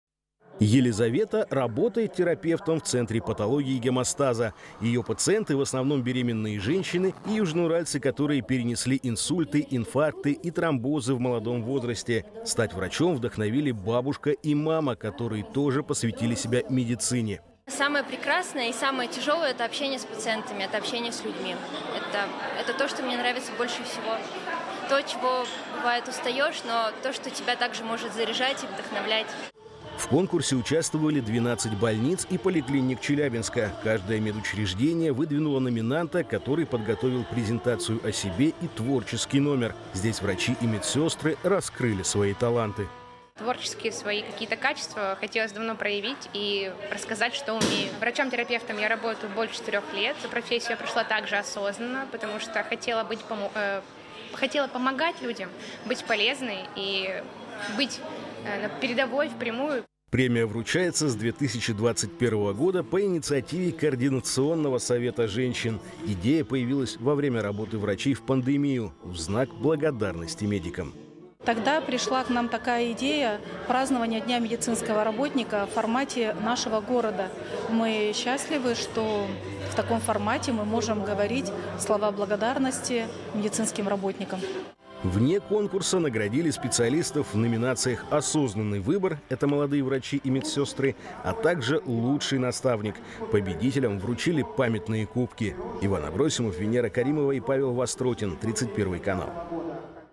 Отличные специалисты и творческие люди. В Челябинске наградили медицинских работников
Челябинским медикам вручили премию "Люди в белых халатах". Ее ежегодно вручают в преддверии профессионального праздника. Кто получил награды в этот раз? Все подробности далее.